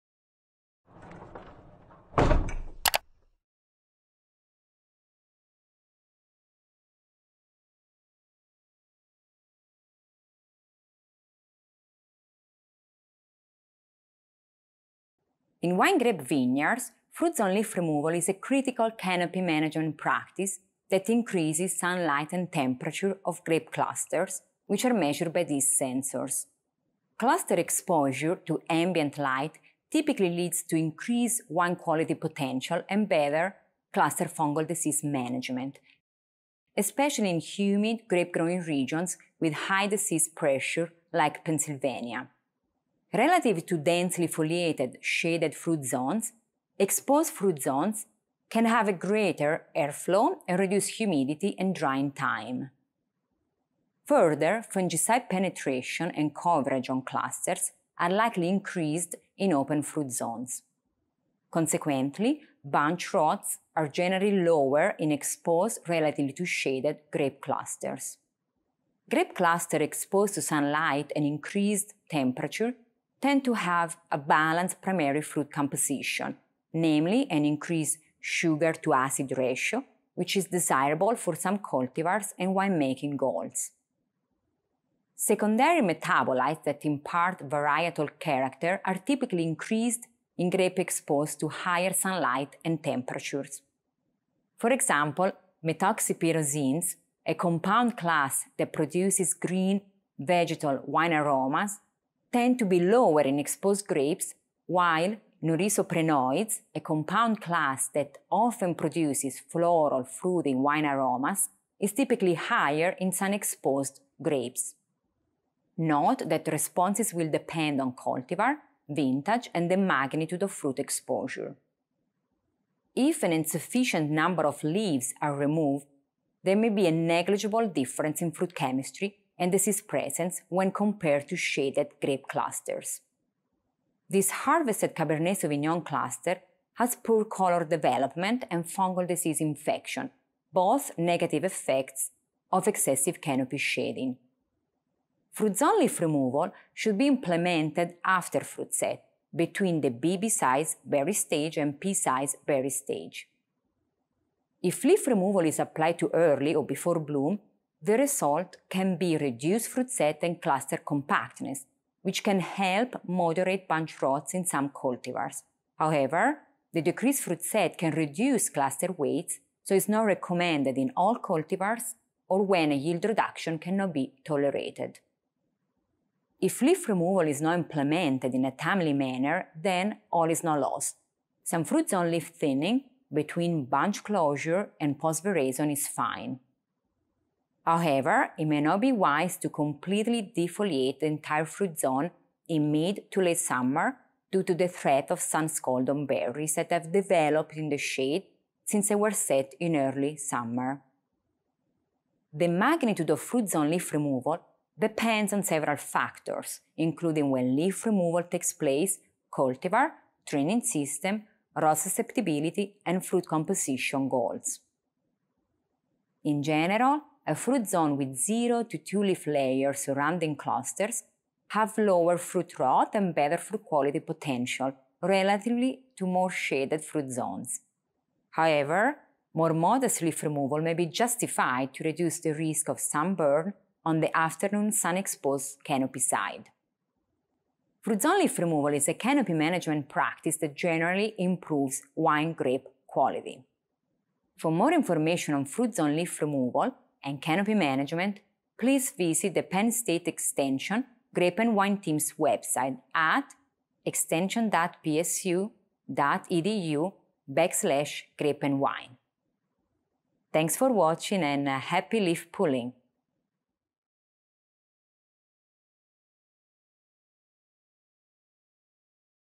Grapevine Fruit Zone Leaf Removal
Learn the fundamentals of grapevine fruit zone leaf removal, including why it is worth implementing, when it should be implemented, and the targeted range of fruit exposure.